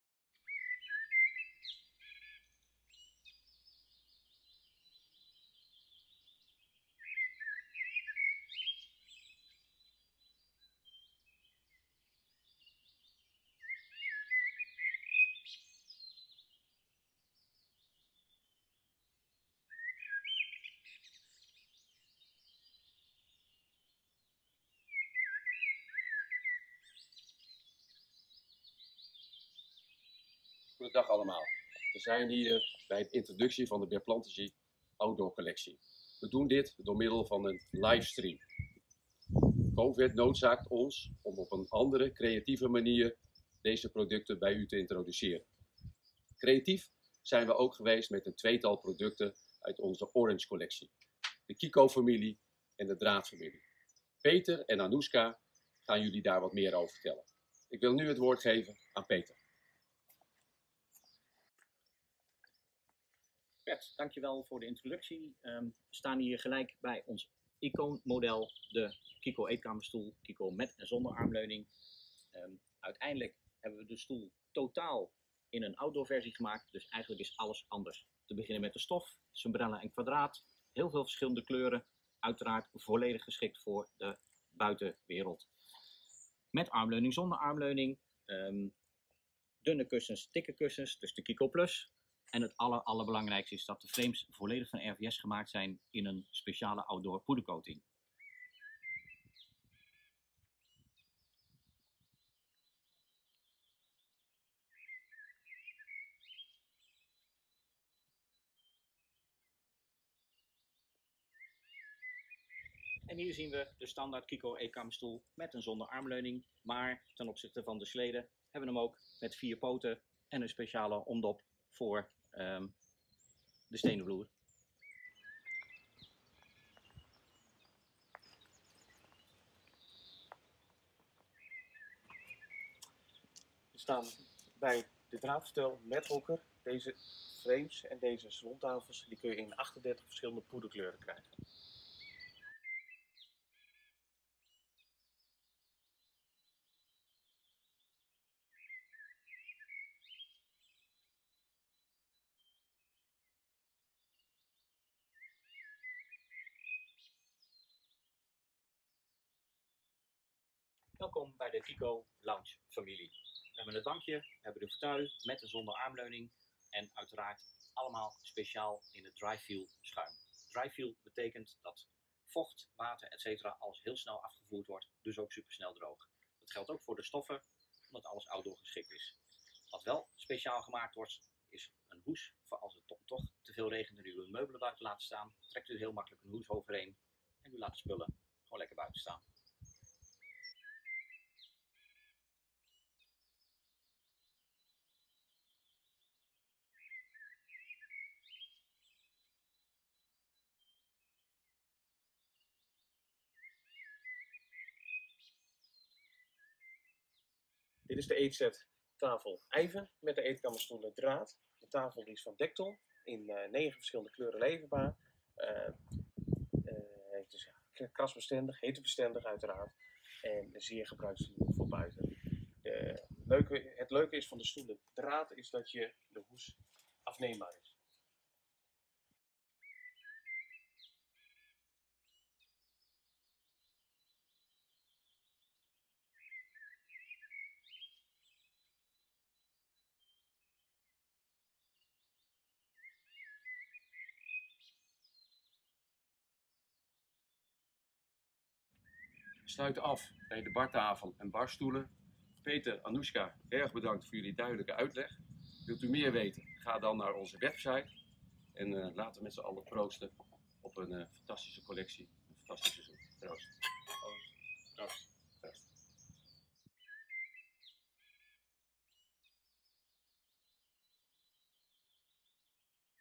Lancering Plantagie Outdoor
Kijk de live-lancering van Plantagie Outdoor terug!

Deze korte video opgenomen in De Tuinen van Appeltern biedt een impressie van onze nieuwste collectie, gebaseerd op de meest populaire designs van bert plantagie.

Dankzij Plantagie Outdoor is het nu mogelijk om buiten te tafelen met hetzelfde comfort als binnen. Helemaal in uw eigen stijl, dankzij het ruime, kleurrijke assortiment dat tientallen stof- en laktinten omvat.